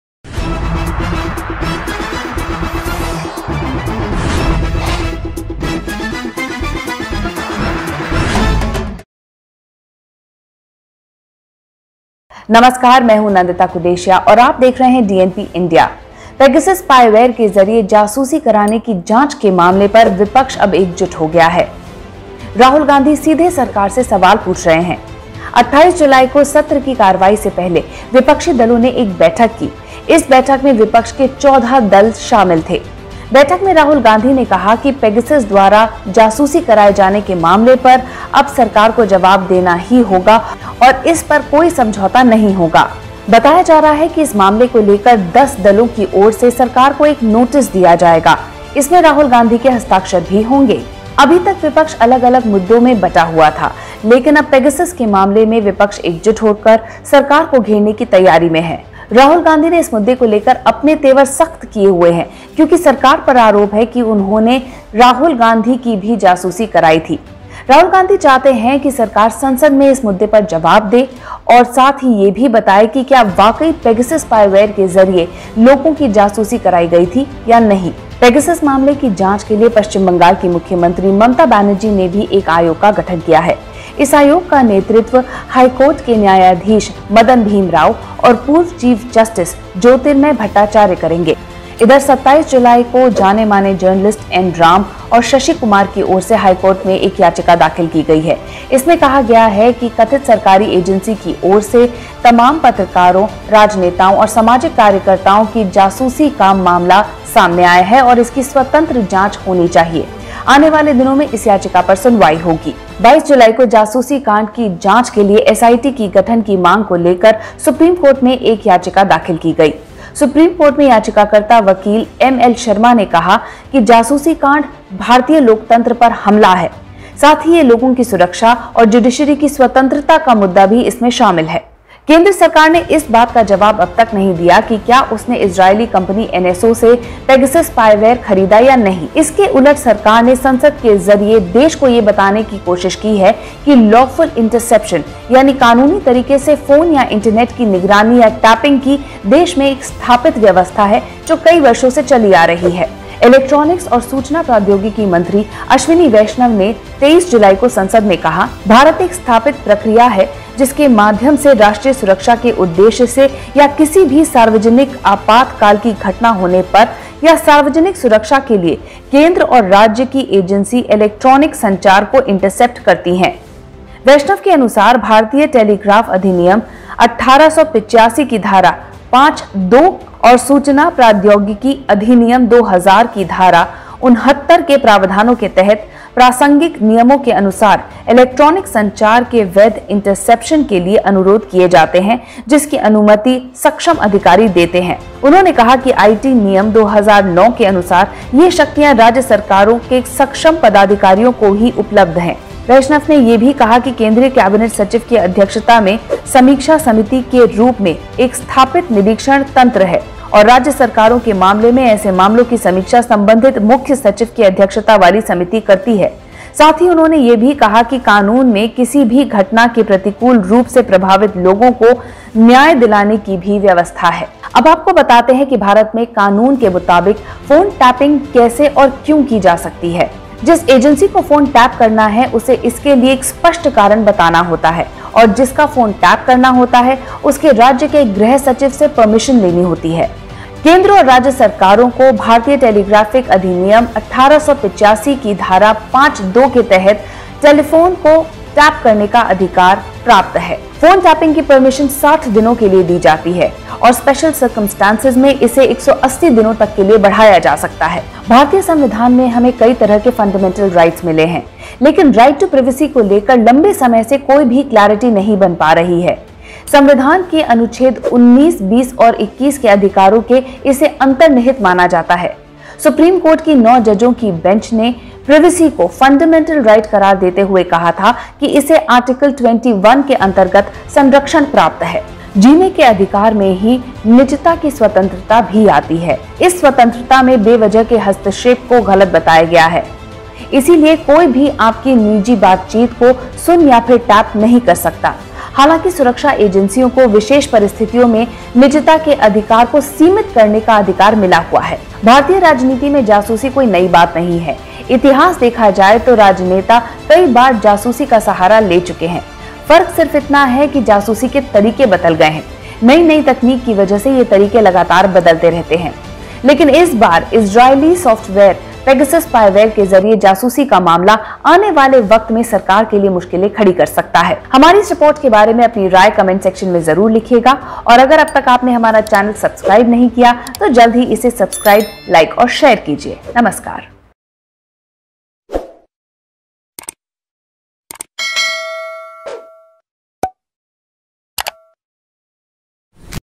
फ़ोन टैपिंग पर क्या कहता है भारत का कानून & कब-कब आया है सियासी भूचाल || DNP INDIA
फ़ोन टैपिंग पर क्या कहता है भारत का कानून & कब-कब आया है सियासी भूचाल